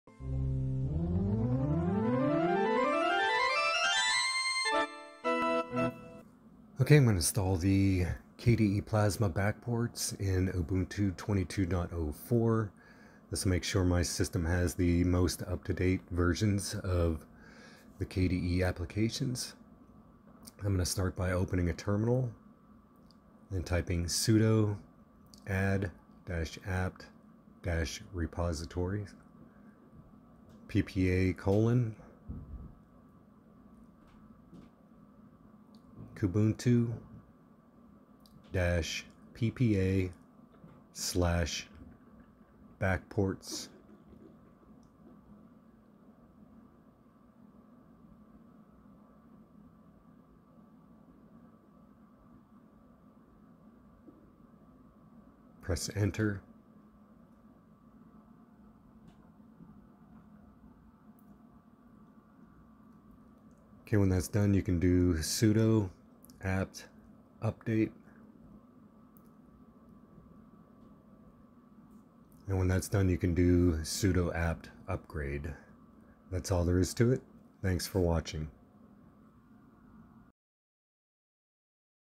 Install KDE Plasma Backports PPA In Ubuntu 22.04
If you installed KDE Plasma Desktop from Ubuntu's software store, you can this ppa to ensure all your KDE apps are the most current version.